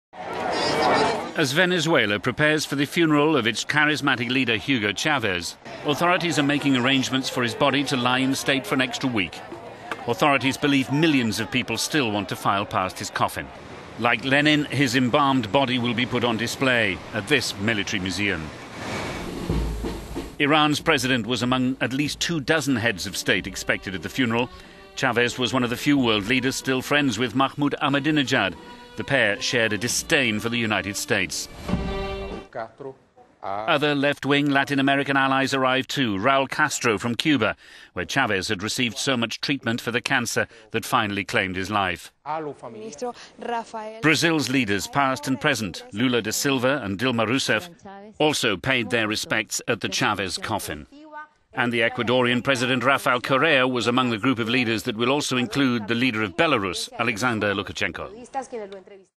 Chavez allies in Venezuela for funeral
As Venezuela prepares for the funeral of its charismatic leader Hugo Chavez, authorities are making arrangements for his body to lie in state for an extra week.

Authorities believe millions of people still want to file past his coffin. Like Lenin, his embalmed body will be put on display.

Iran's president was among at least two dozen heads of state expected at the funeral. Chavez was one of the few world leaders still friends with Mahmoud Ahmadinejad.The pair shared a disdain for the United States.

Other left-wing Latin American allies arrived too - Raul Castro from Cuba, where Chavez had received so much treatment for the cancer that finally claimed his life.

Brazil's leaders past and present, Lula da Silva and Dilma Rousseff also paid their respects at the Chavez coffin.

And the Ecuadorean president Rafael Correa was among the group of leaders that will also include the President of Belarus Alexander Lukachenko.